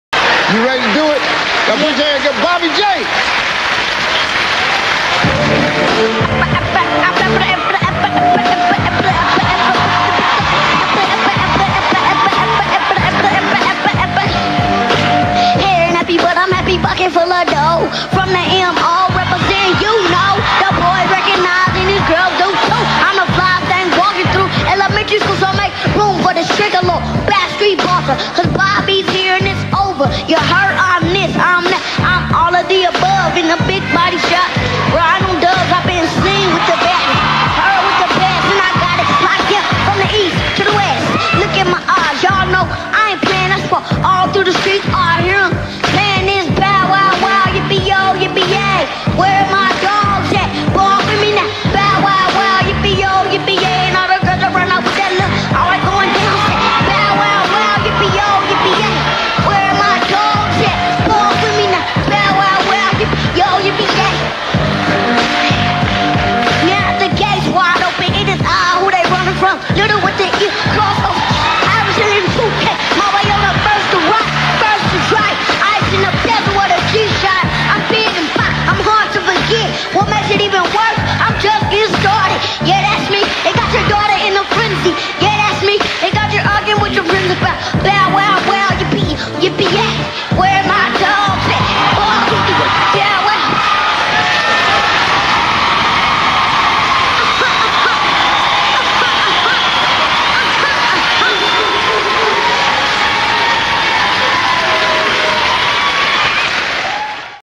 Some little MC
My friend Anna just sent me this video, he is quite funny :) If someone know his name, please let me know!